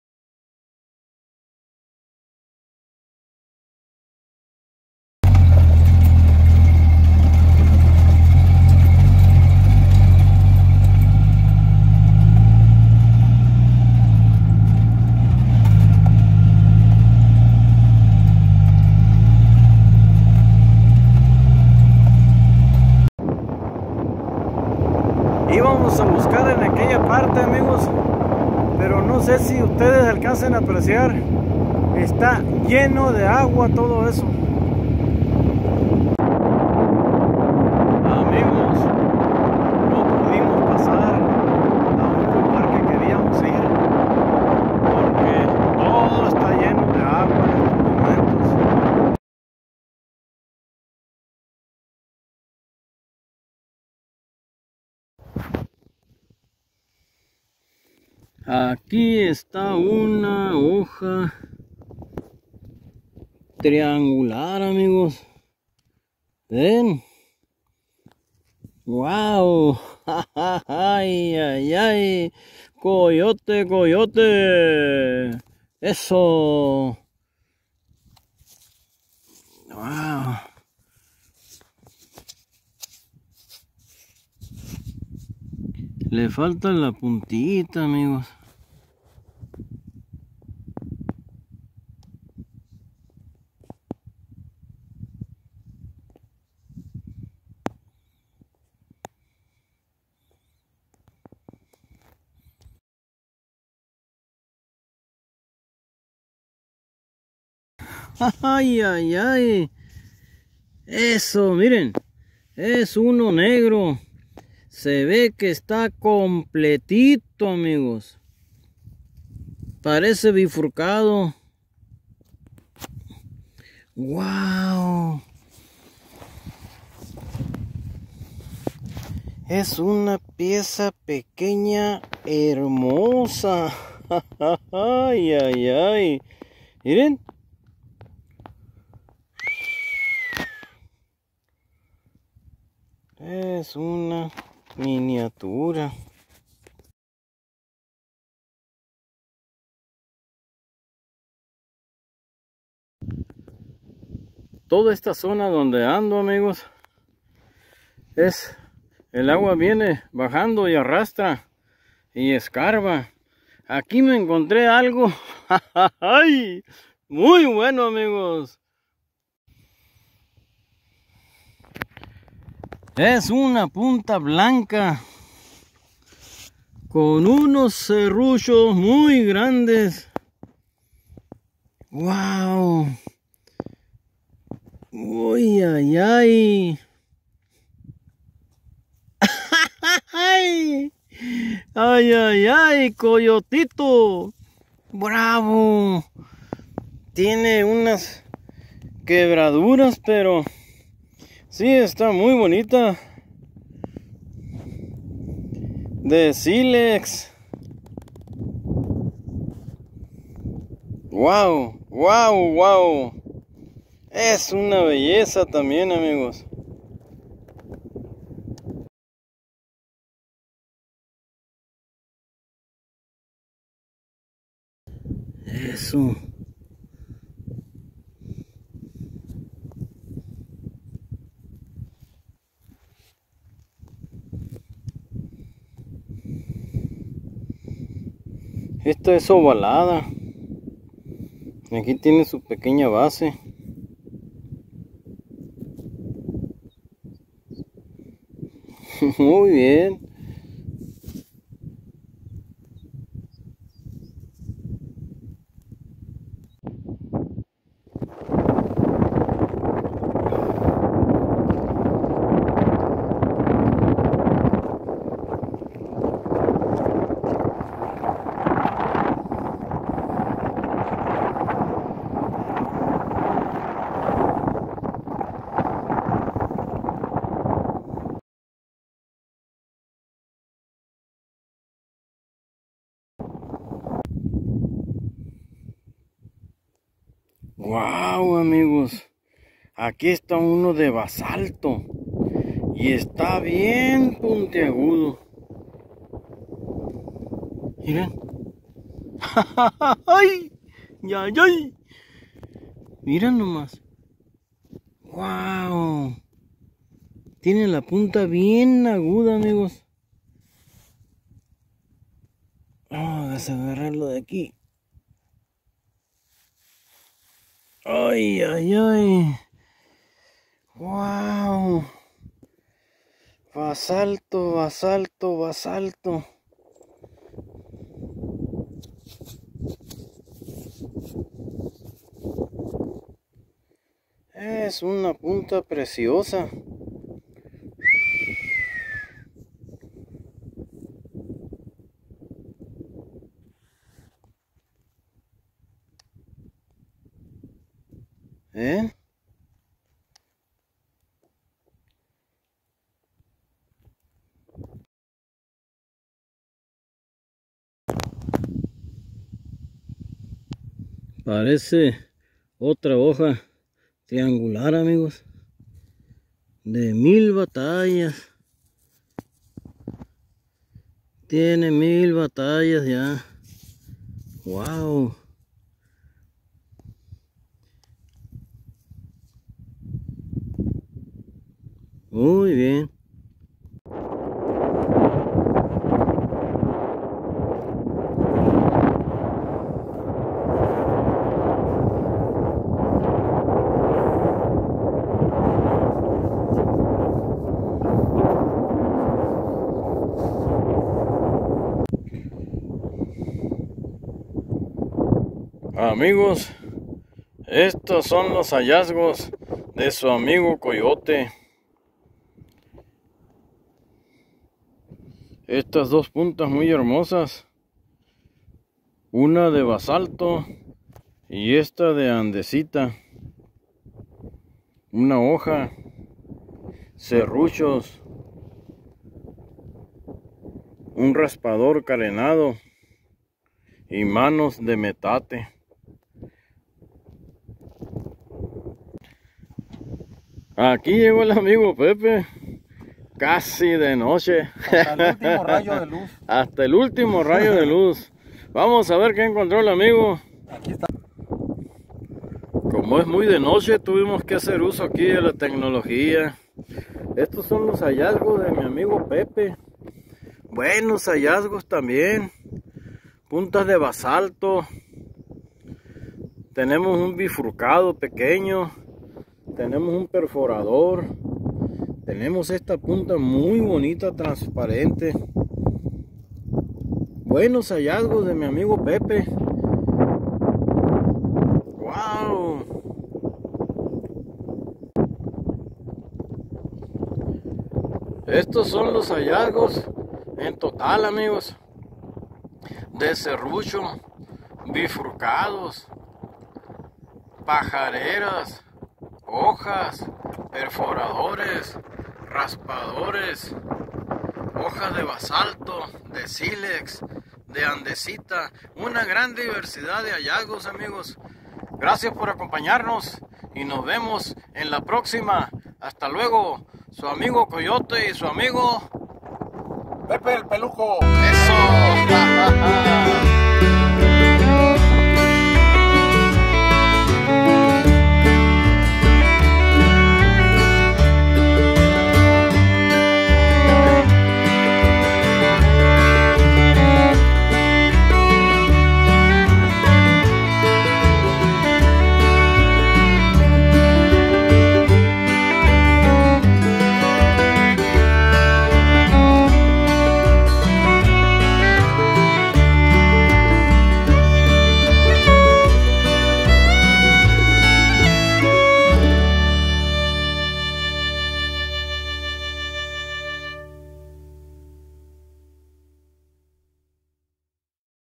81 ARROWHEAD HUNTING Wow great diversity of beautiful finds Final photograph
In the previous video we showed you the findings that my friend Pepe and I found in the morning in the great plains after a rain. In this video we will show you the findings we made in the afternoon.